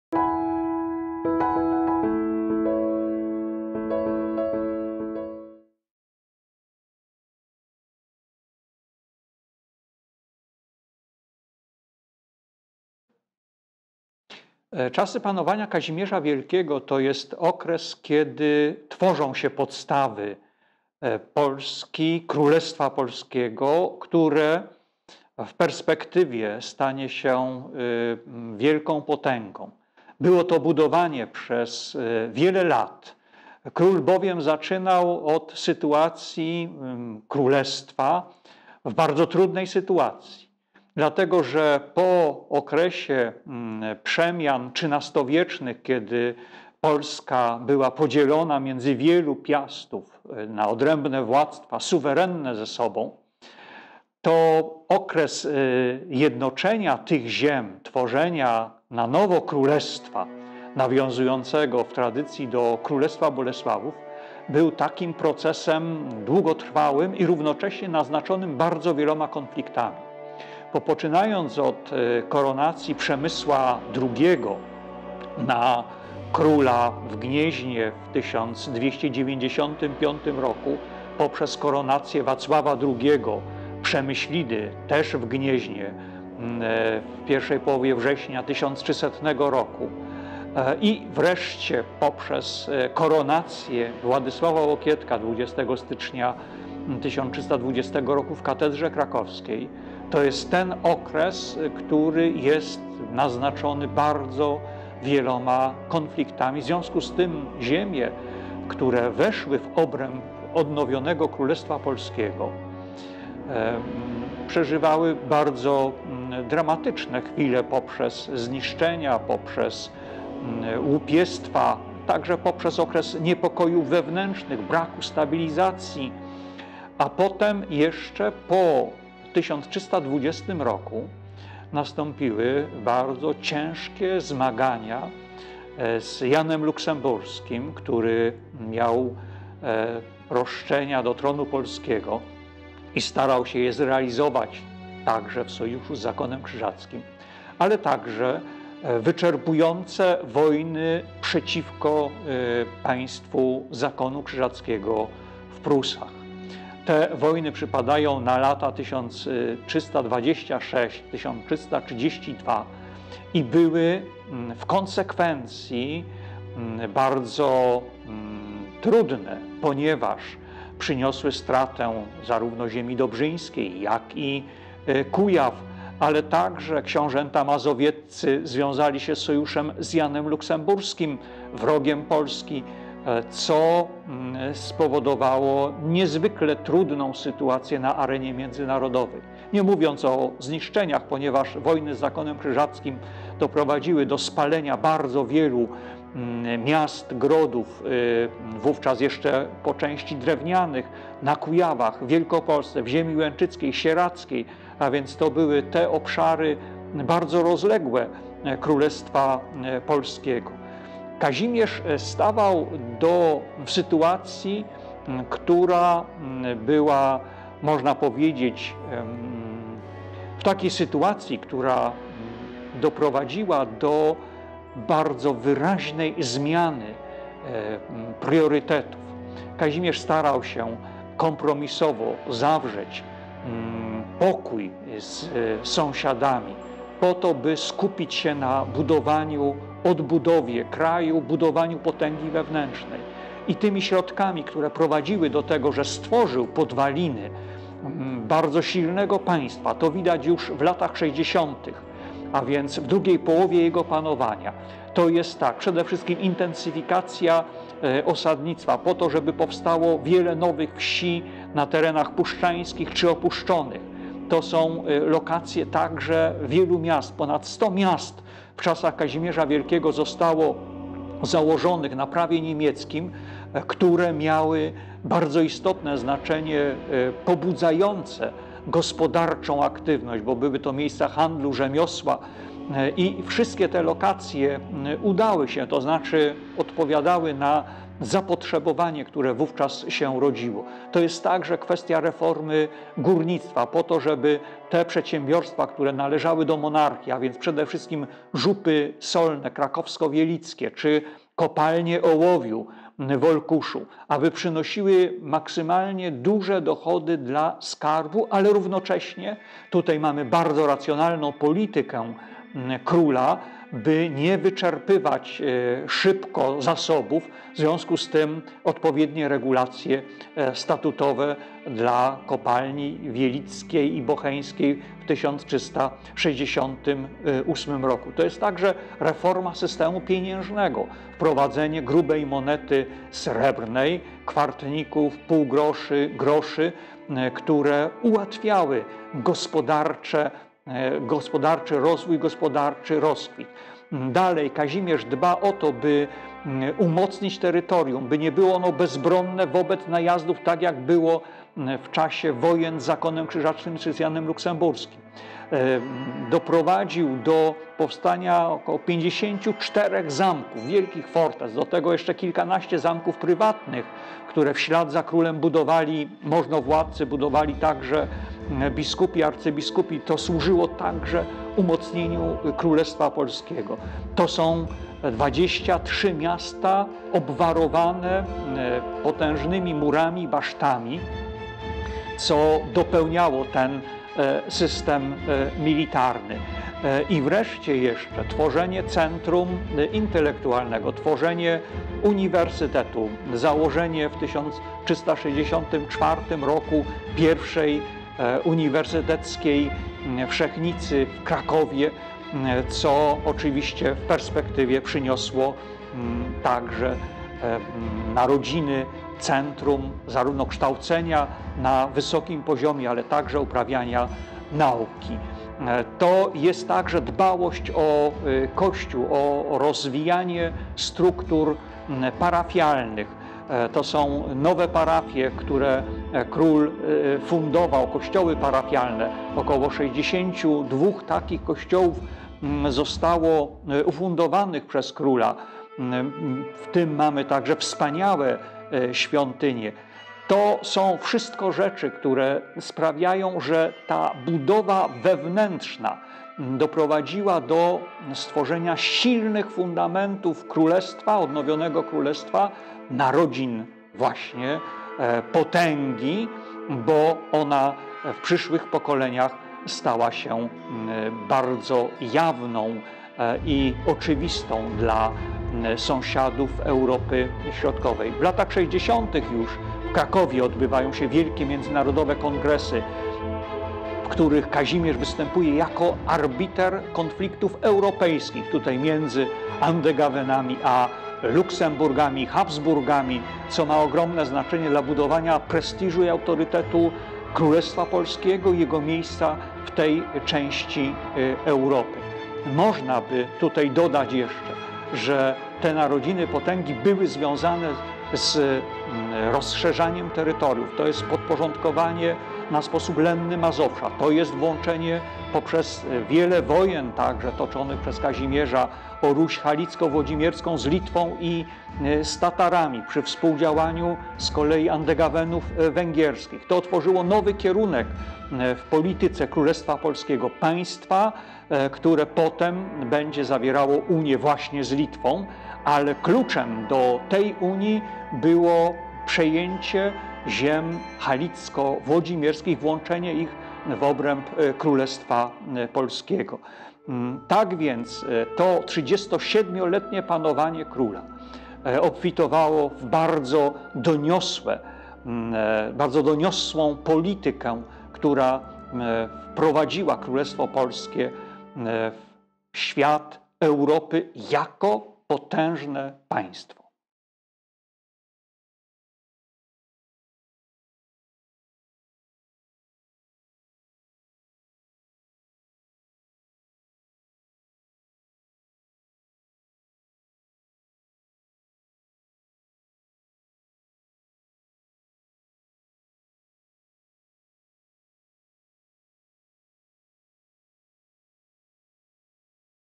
Narodziny potęgi. Dziedzictwo Kazimierza Wielkiego. || Prof. Krzysztof Ożóg
Naprawdę jest czym się szczycić, jeśli chodzi o polską historię. Już Bolesław Chrobry (967–1025) stworzył bardzo silne i rozległe państwo. Ponad sto lat po nim przyszedł okres osłabienia, zwany rozbiciem dzielnicowym, ale pamiętajmy, że było to wówczas zjawisko powszechne w całej Europie. Ponownego zjednoczenia państwa dokonuje dzielny przedstawiciel Piastów, książę kujawski Władysław Łokietek, wieńcząc swoje wysiłki królewską koronacją w 1320 r. w katedrze wawelskiej. Jego syn Kazimierz (1310–1370), wychowany w duchu silnego przywiązania do Ojczyzny i Chrystusowej wiary, kontynuował dzieło ojca. Po śmierci Władysława wieńczy on swe skronie koroną 25 kwietnia 1333 r. i rozpoczyna długie, owocne, a przy tym barwne panowanie.
Barwnie odpowiada o tym wybitny mediewista i biograf króla Kazimierza Wielkiego, prof. Krzysztof Ożóg.

Partner Publikacji: Alior Bank

Muzyka w filmie:
Remigiusz Petrykowski

Dofinansowano ze środków Ministerstwa Kultury i Dziedzictwa Narodowego (z Funduszu Promocji Kultury).